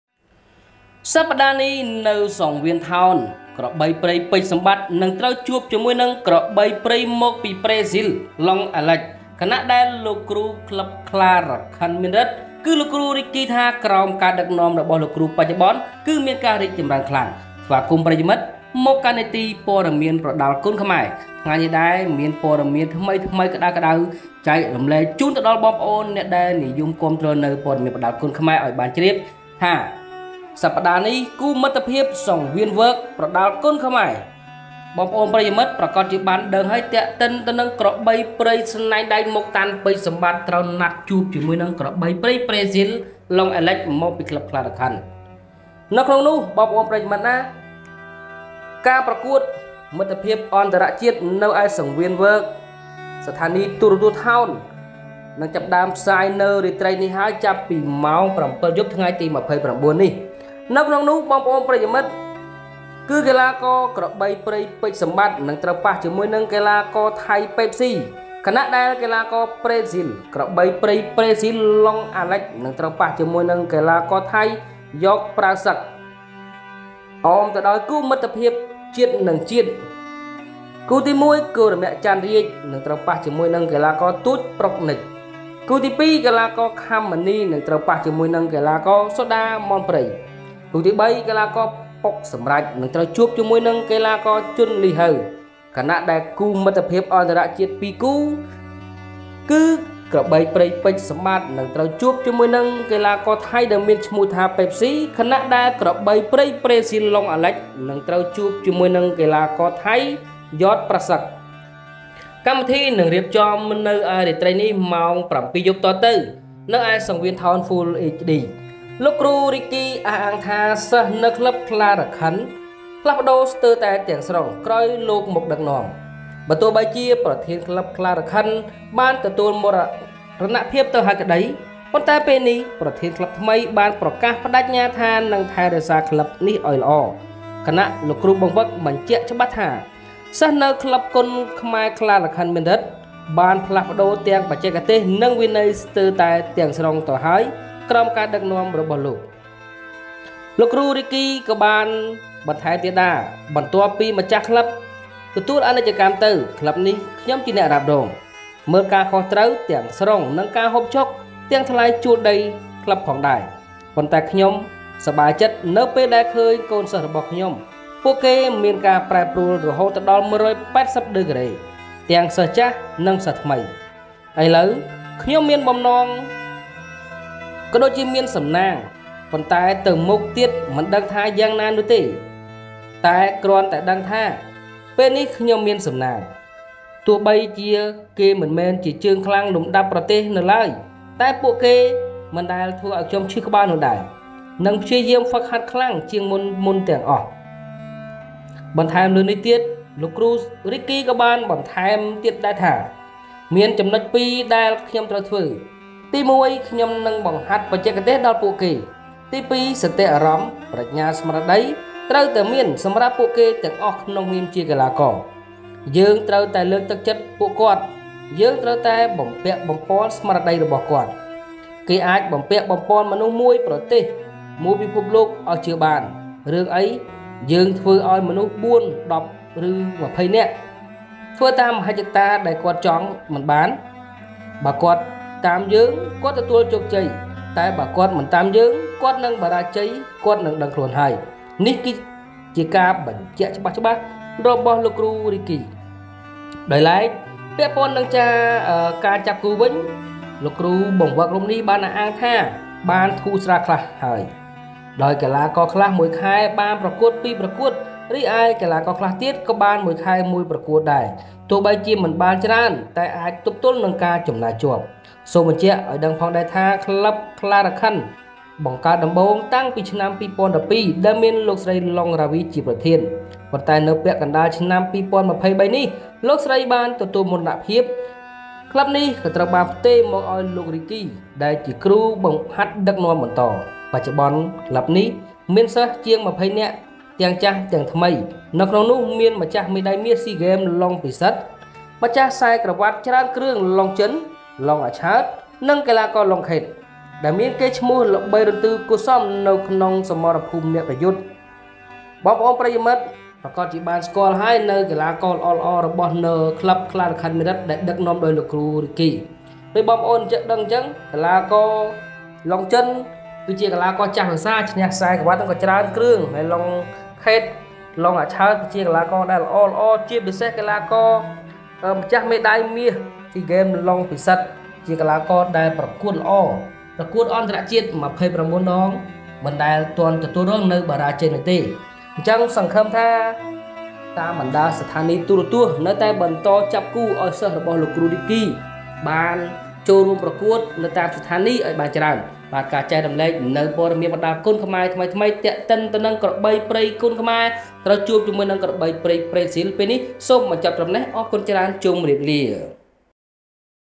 ជួបហើយ🔥ក្របីព្រៃ ពេជ្រសម្បតិ្ត ជួបក្របីព្រៃប្រេស៊ីល Alex នៅសង្វៀនថោន លោកគ្រូ រីកគី ដាក់ចំៗថា..
ជួបហើយ🔥ក្របីព្រៃ ពេជ្រសម្បតិ្ត ជួបក្របីព្រៃប្រេស៊ីល Alex នៅសង្វៀនថោន លោកគ្រូ រីកគី ដាក់ចំៗថា..ក្រោមការគ្រប់គ្រងរបស់លោកគ្រូ បច្ចុប្បន្នរីកចម្រើន១៨០ដឺក្រេ..